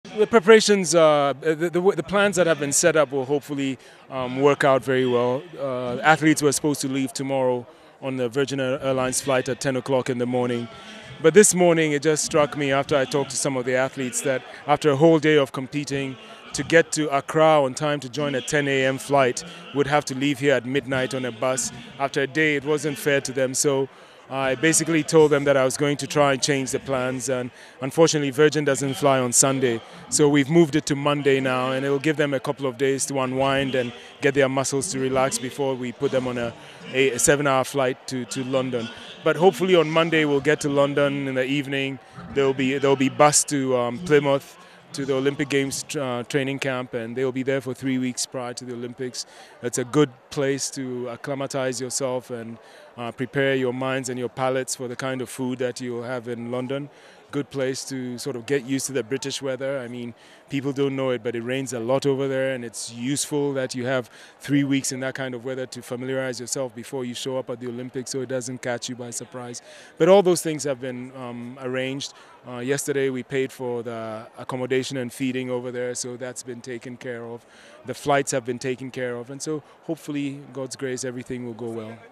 Professor Francis Dodoo on Joy Sports
Professor Francis Dodoo, interviewed on Joy Sports